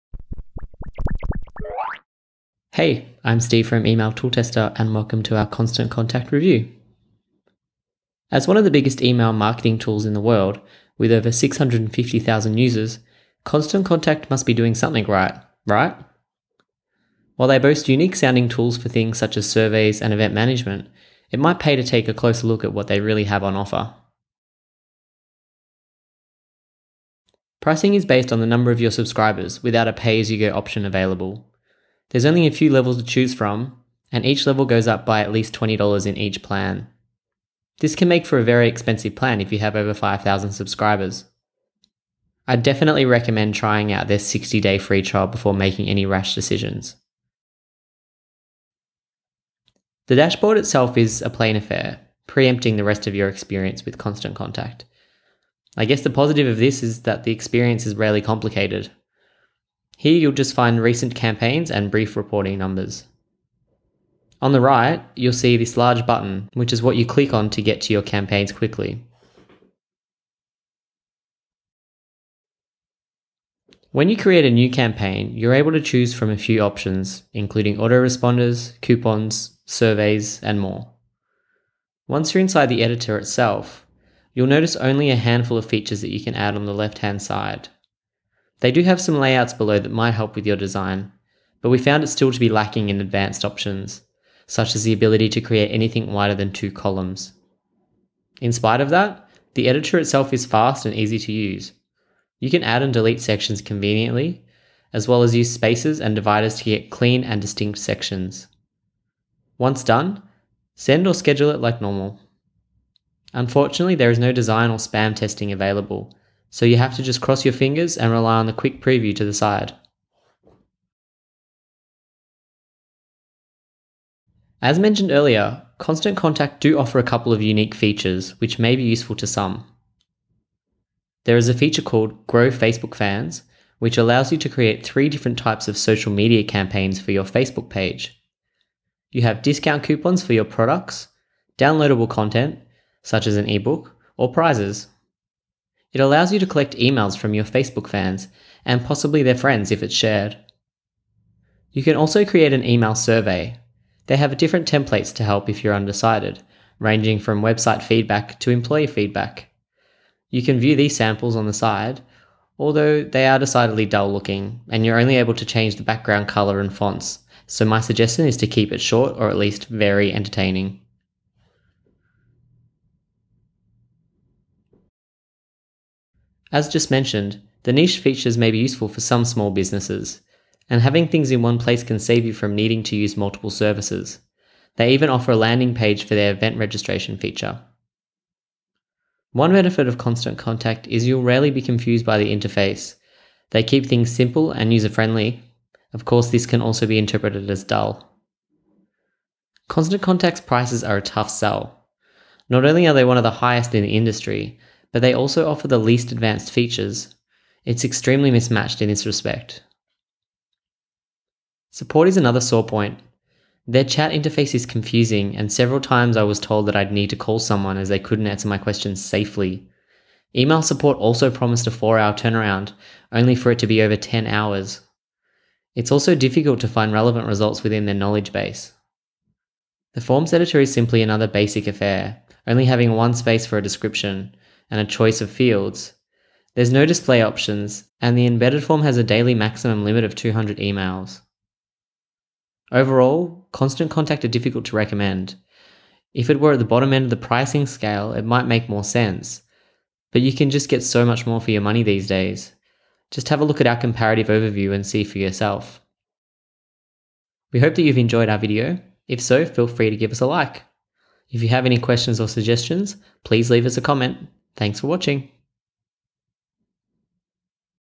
Constant Contact Review - The Pros & Cons of the Newsletter Tool
Learn all about Constant Contact's strengths and weaknesses in this video. We'll show you how to create a newsletter, how their niche social features work and much more.

Contents:
0:00 Intro
0:26 Pricing
0:50 Dashboard
1:13 Editor
2:04 Features
2:54 Conclusion